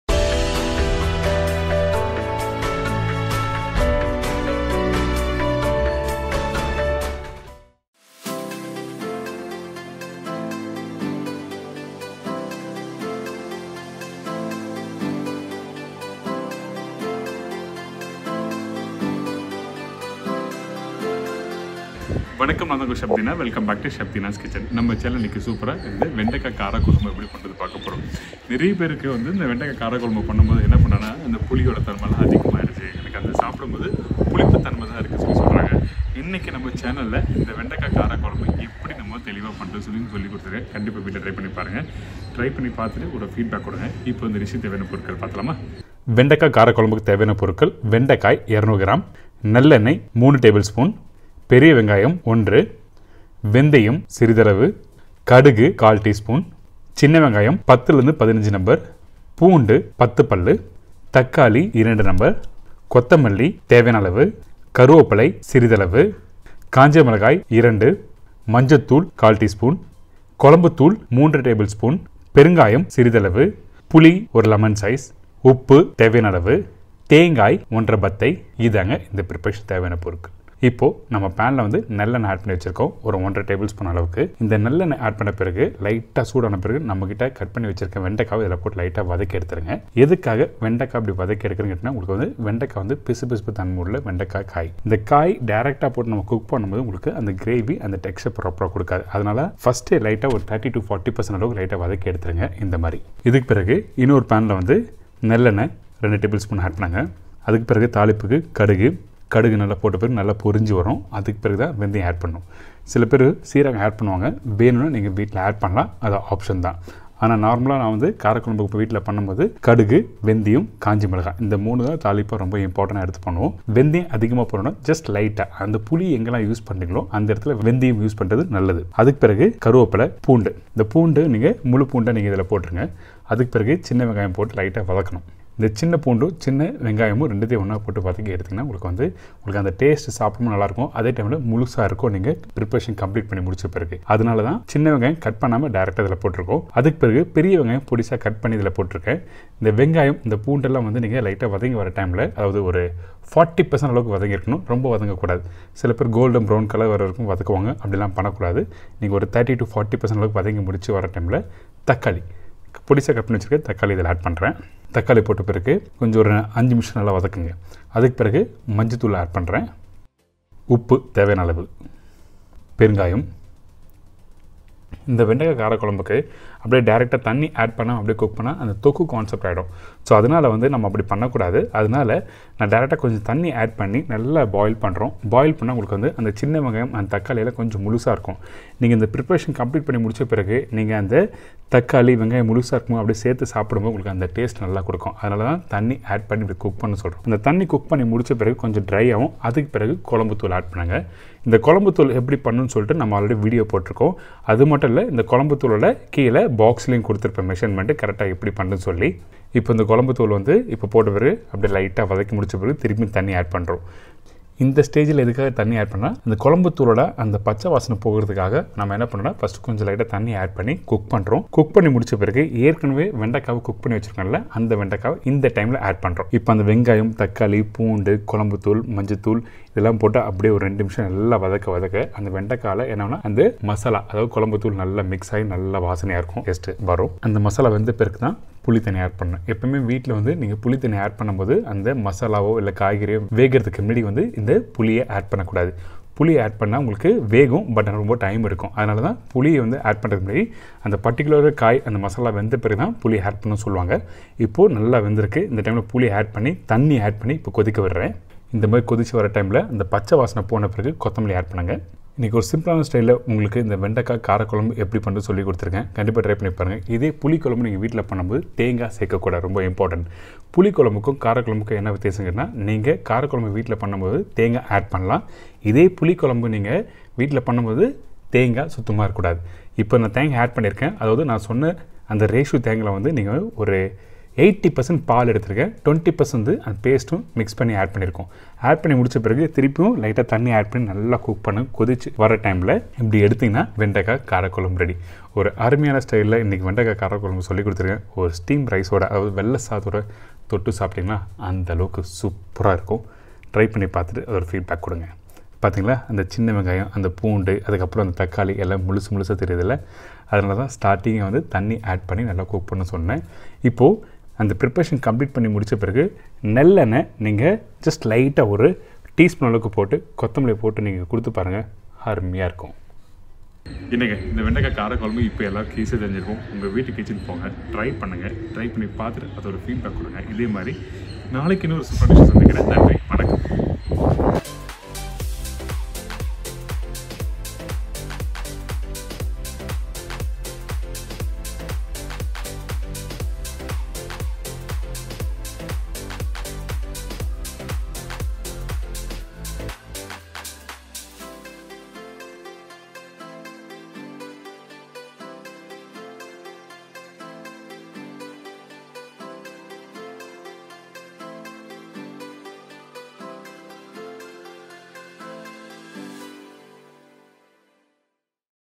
Vendaikai kaara Kozhambu | Lunch Kozhambu Recipes | CDK #225 | Chef Deena's Kitchen
Hi All!
Welcome to chef Deena’s Kitchen!
Today, In this video you will learn how to make Kaara Kozhambu. This is a very famous recipe in Tamil Nadu, kalyana Kaara Kozhambu is everyone’s favourite, Will post a video soon. I have already posted Kozhambu thool on our channel! Please check out the video and make this recipe using Kozhambu thool. Do try and share your feedback in comments below

வெண்டைக்காய் / Lady’s Finger - 200 Gms
நல்லெண்ணெய் / Sesame Oil - 3 TBSP
வெங்காயம் / Onion - 1
வெந்தயம் / Fenugreek Seeds
கடுகு / Mustard Seeds - 1/4 Spoon
சின்ன வெங்காயம் / Small Onion - 10 to 15 Nos
பூண்டு / Garlic - 10 Pieces
தக்காளி / Tomato - 2
கொத்தமல்லி / Coriander Leaves
கருவேப்பிலை / Curry Leaves
காய்ந்த மிளகாய் / Dry Chilli - 2Nos
மஞ்சள் தூள் / Turmeric Powder
குழம்பு தூள் / Kulambu Powder - 3 TBSP
பெருங்காயம் / Asafoetida
புளி / Tamarind
உப்பு / Salt
தேங்காய் / Coconut - 1 1/2 Pieces

Spice Boxes
wooden Box
Frying Pan Cast Iron
Multipurpose Tray
Cooking Laddle
Wooden Spatula
Women’s Apron
Kitchen Apron

Editing: Jagadish.V